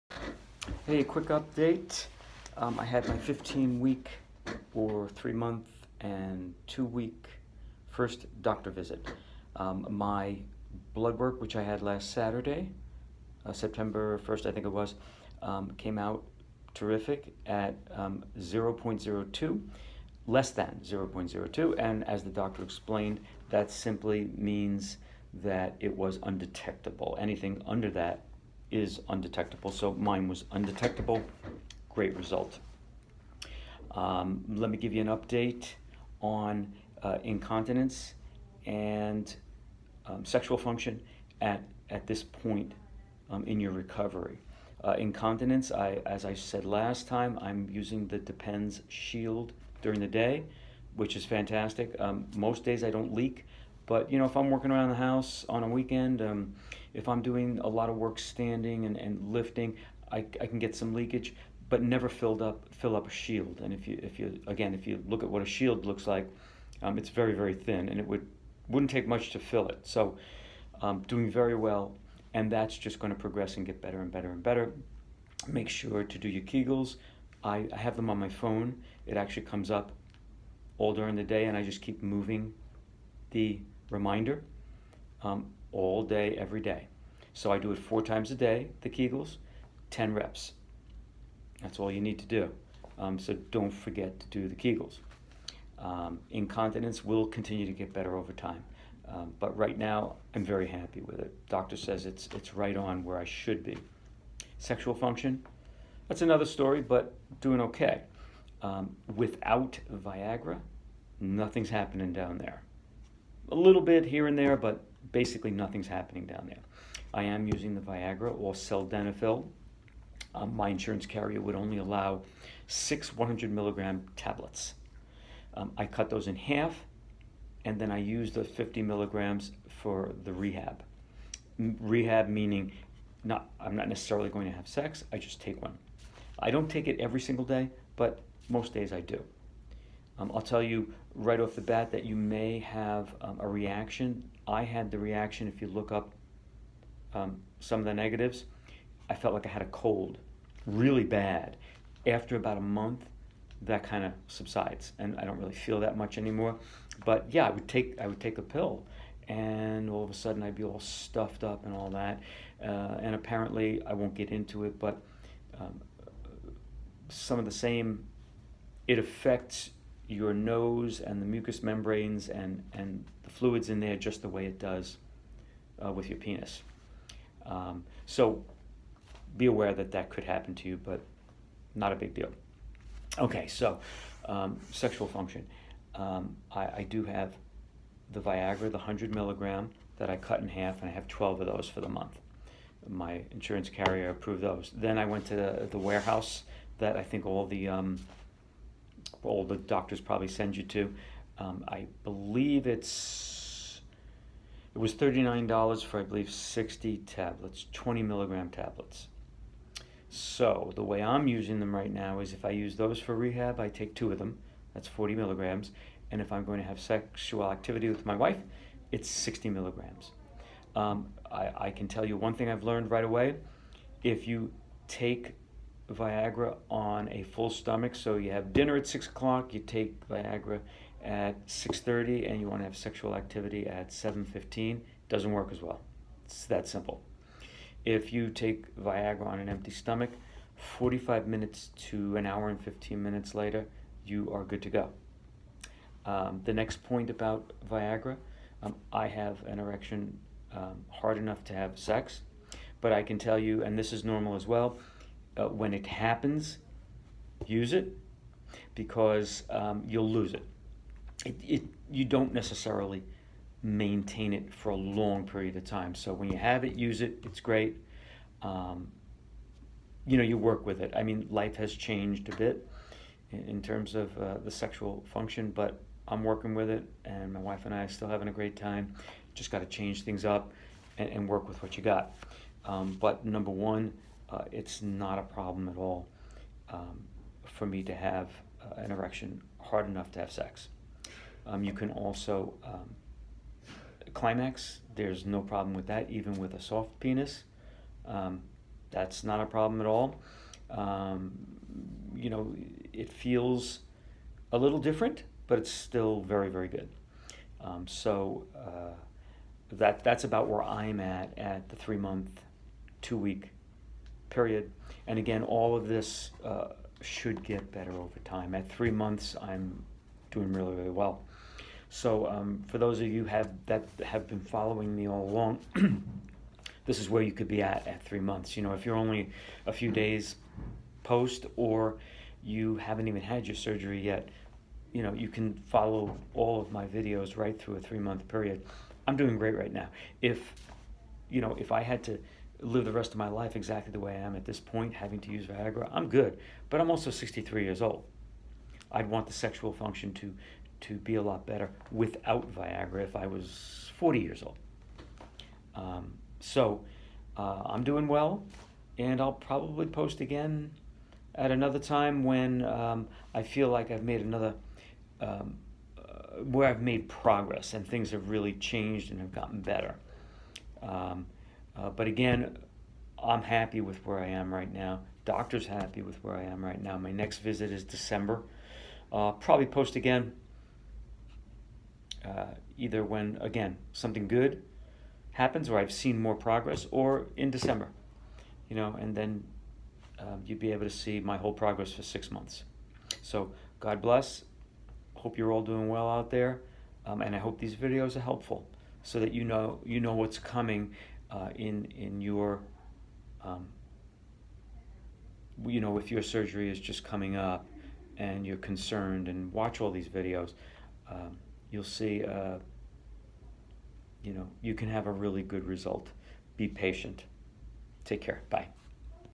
Prostatectomy 15 weeks post surgery - incontinence and sexual function
First follow-up doctor visit and update on incontinence and sexual function